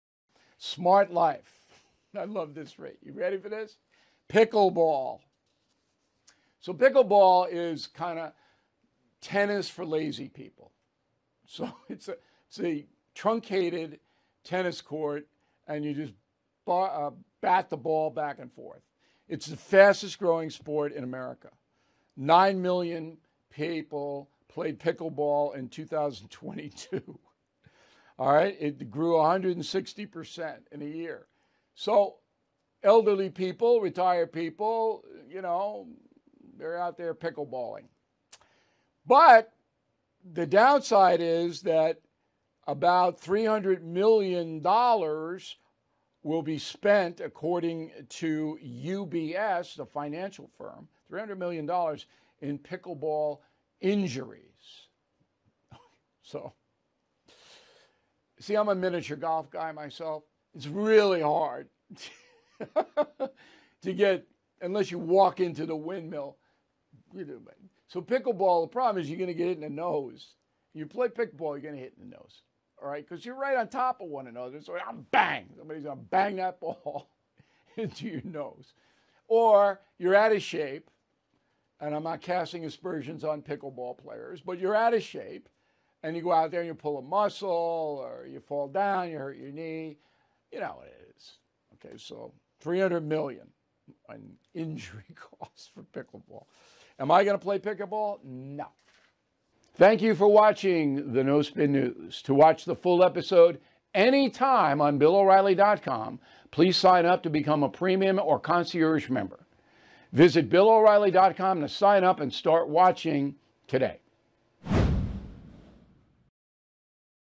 Pickleball May Be Hazardous to Your Health
On the Smart Life segment Bill looks at the fast-growing sport of pickleball and the astonishing amount of pickleball injuries.